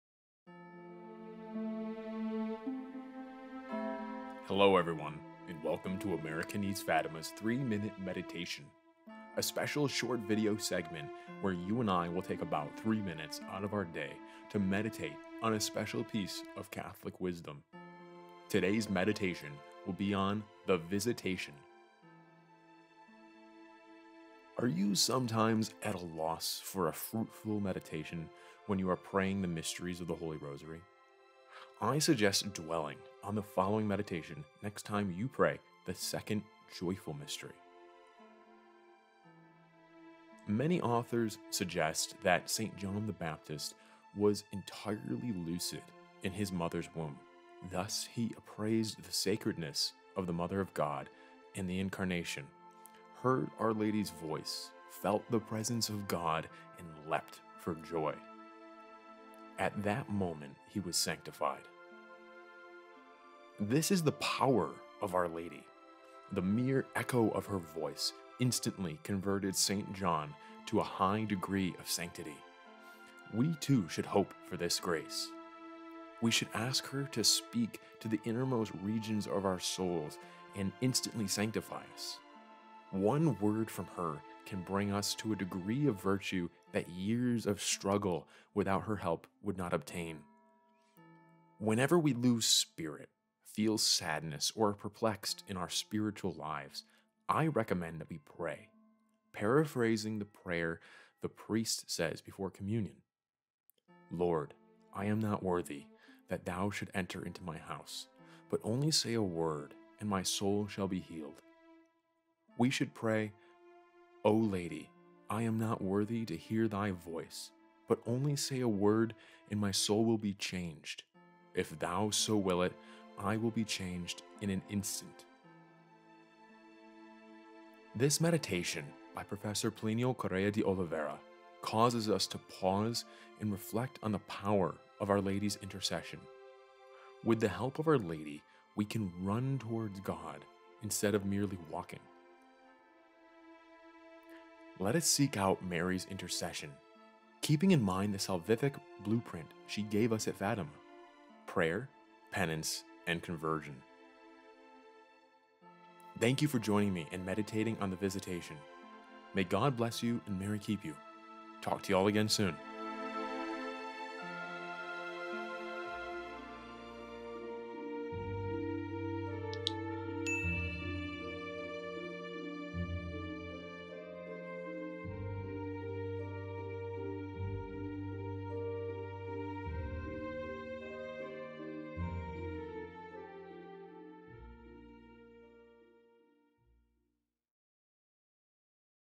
Mystery of the Visitation — Catholic Prayers to Our Lady
Hello everyone, and welcome to America Needs Fatima’s 3-minute Meditation, a special short-video segment where you and I will take about three minutes out of our day to meditate on a special piece of Catholic wisdom. Today’s meditation will be on The Visitation.

Are you sometimes at a loss for a fruitful meditation when you are praying the mysteries of the Holy Rosary? I suggest dwelling on the following meditation next time you pray the second Joyful Mystery.

“[Many authors] suggest that [Saint John the Baptist] was entirely lucid in his mother’s womb. Thus, he appraised the sacredness of the Mother of God and the Incarnation, heard Our Lady’s voice, felt the presence of God and leapt for joy. At that moment, he was sanctified.
This is the power of Our Lady. The mere echo of her voice instantly converted Saint John to a high degree of sanctity. We too should hope for this grace.

We should ask her to speak to the innermost regions of our souls and instantly sanctify us. One word from her can bring us to a degree of virtue that years of struggle, without her help, would not obtain.

Whenever we lose spirit, feel sadness or are perplexed in our spiritual lives, I recommend we pray, paraphrasing the prayer the priest says before communion: ‘Lord I am not worthy that Thou should enter into my house, but only say a word and my soul shall be healed.’
We should pray: ‘O Lady, I am not worthy to hear thy voice, but only say a word and my soul will be changed. If thou so will it, I will be changed in an instant.’”

This meditation by Professor Plinio Corrêa de Oliveira causes us to pause and reflect on the power of Our Lady’s intercession. With the help of Our Lady, we can run towards God instead of merely walk.

Let us seek out Mary’s intercession, keeping in mind the salvific blueprint she gave us at Fatima: prayer, penance, and conversion.

Thanks again for joining me in meditating on the Visitation. May God bless you and Mary keep you. Talk to you all again soon.